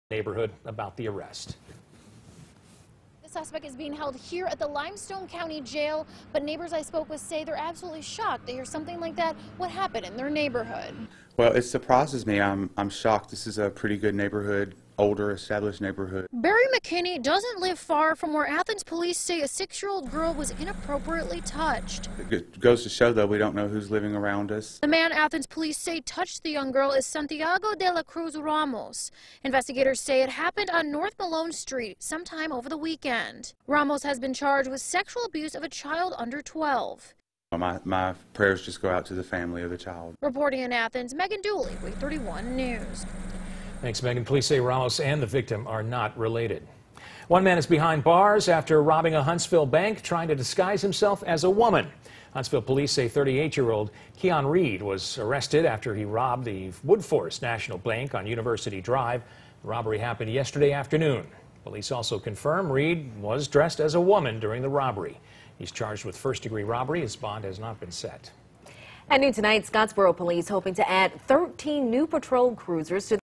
Athens Police Arrest Man On Sex Abuse Charges
Neighbors -- shocked -- as a man in their neighborhood is arrested for inappropriately touching a six year old girl.